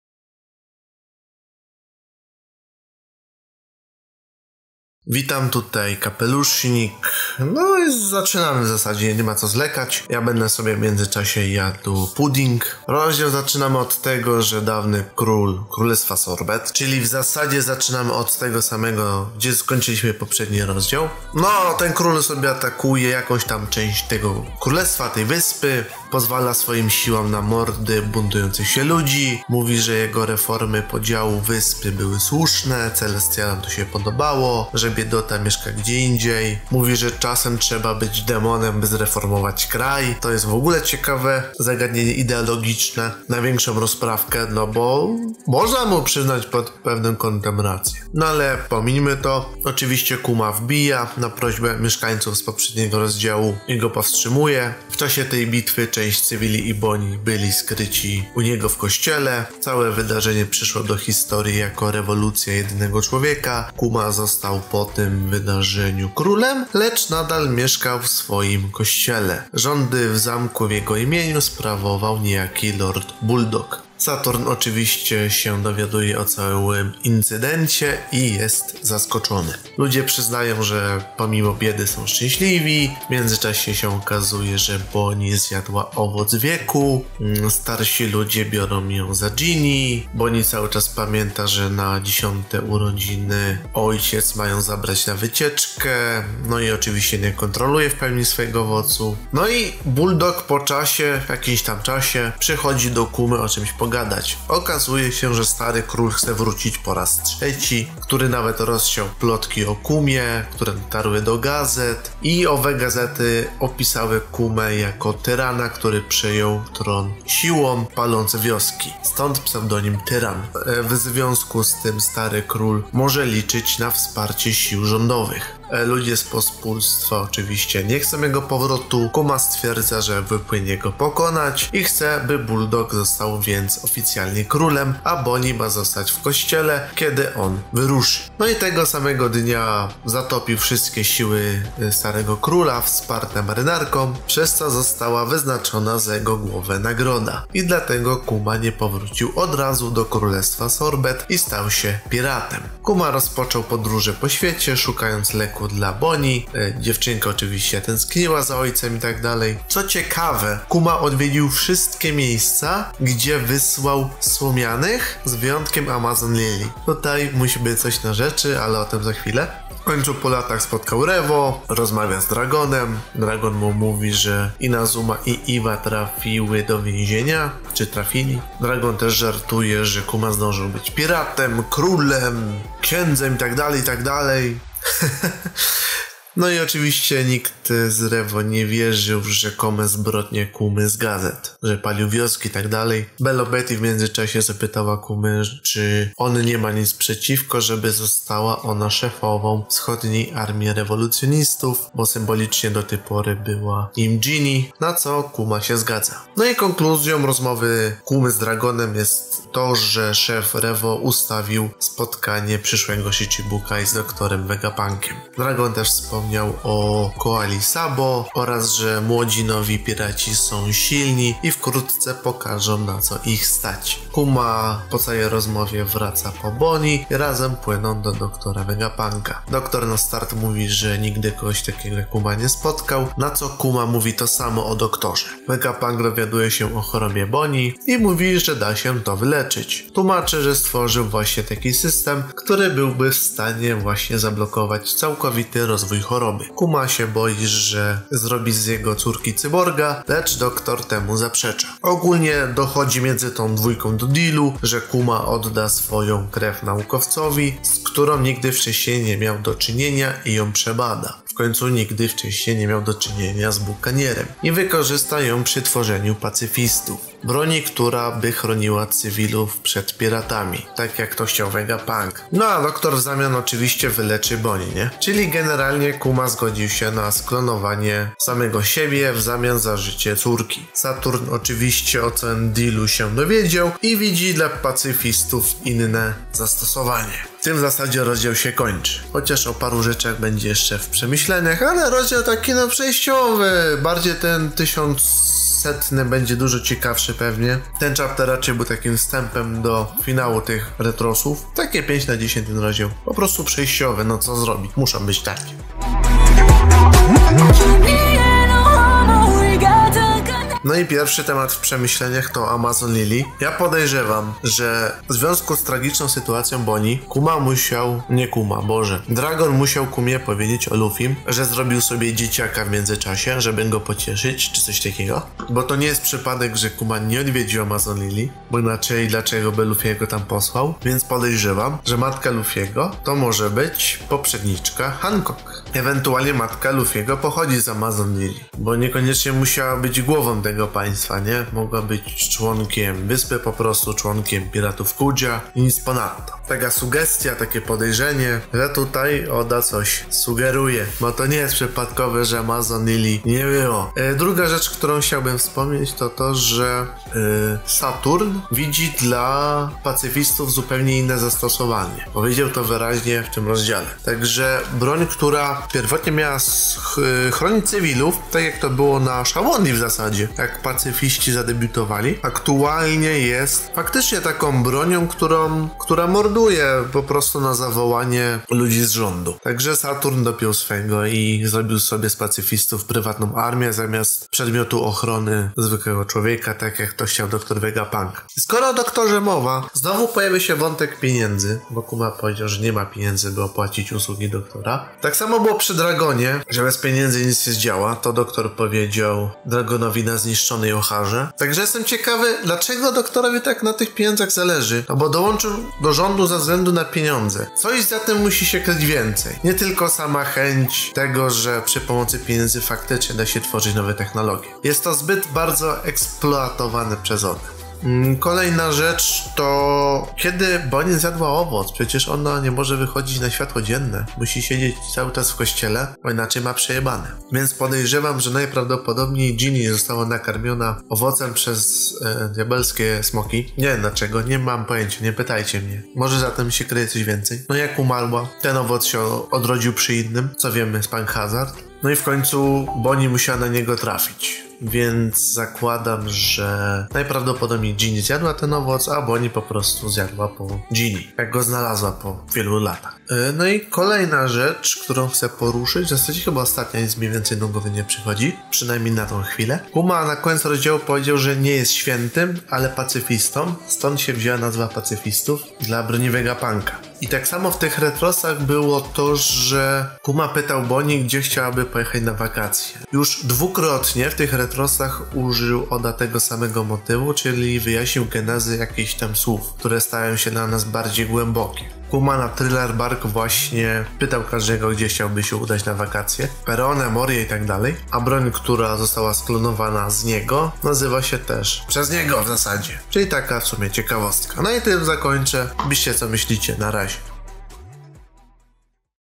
OMÓWIENIE ROZDZIAŁU ONE PIECE #65 ~ KUMA PIRATEM (ROZDZIAŁ 1099)
SCENARIUSZ: KAPELUSZNIK & DESTHER
MONTAŻ: KAPELUSZNIK
NARRACJA: KAPELUSZNIK
TWÓRCA MINIATURKI: KAPELUSZNIK

SPIS:

0:00 - 5:08 OMÓWIENIE 1099
5:08 - 8:19 PRZEMYŚLENIA